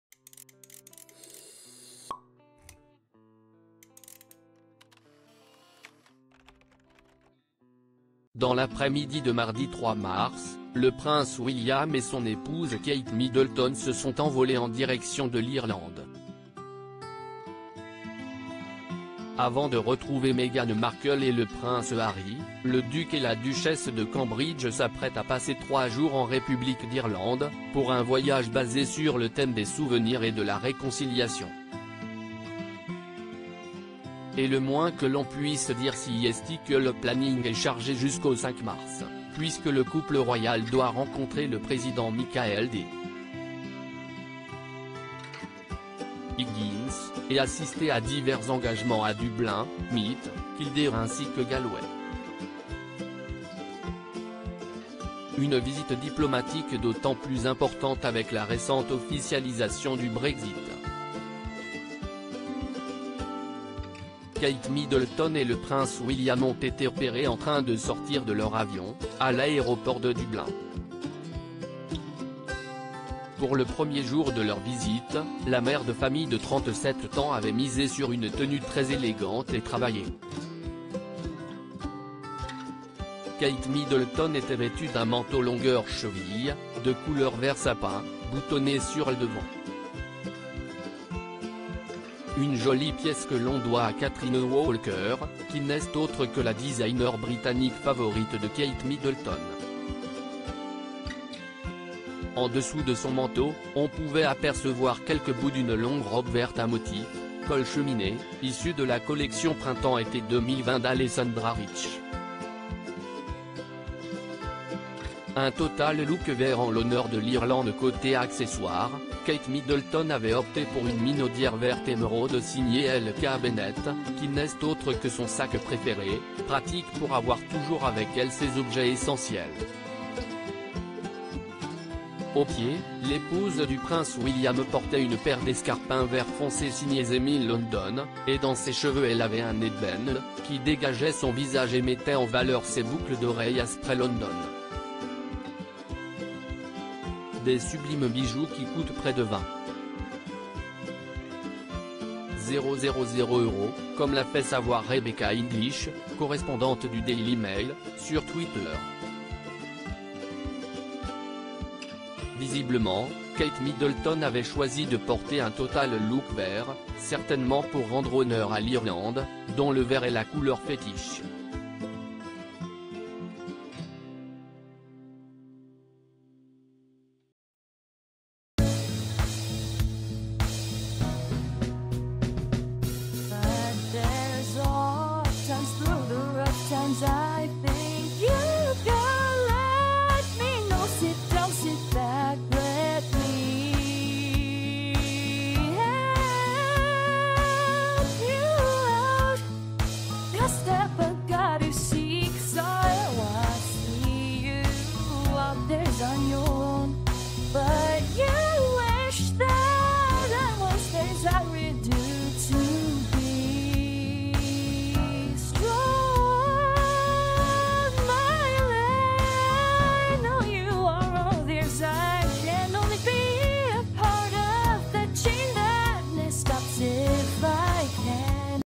Kate Middleton rayonnante en vert pour le premier jour de son voyage en Irlande avec le prince Willi
Kate Middleton rayonnante en vert pour le premier jour de son voyage en Irlande avec le prince William